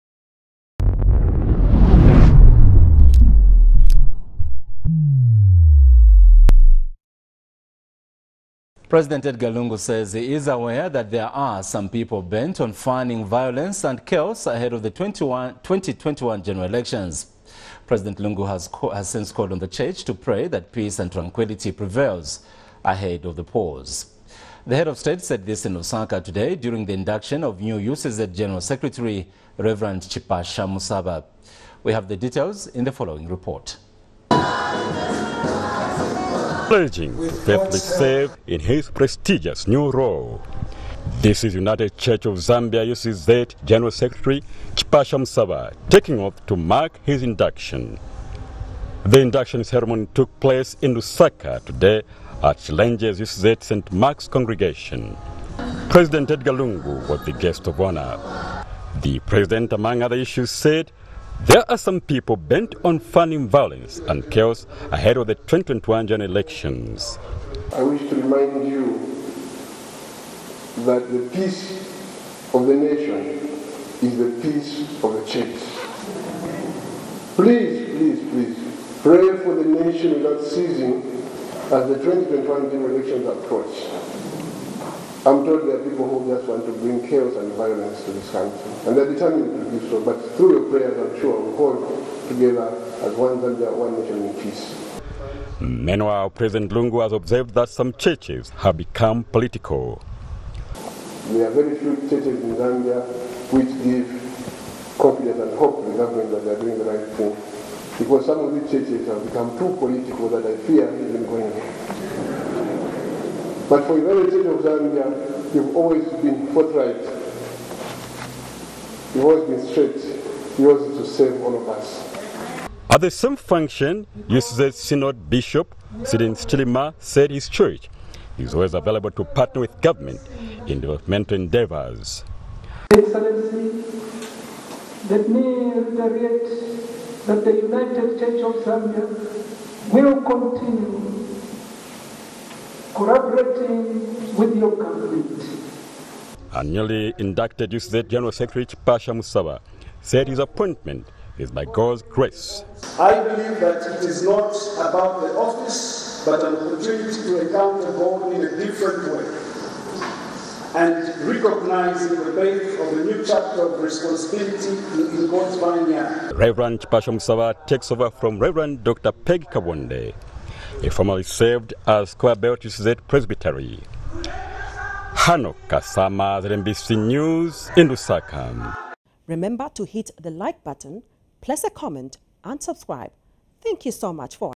ECL AWARE OF OPPOSITION SCHEME
President EDGAR LUNGU says he is aware that there are some people bent on funning violence and chaos ahead of the 2021General Election.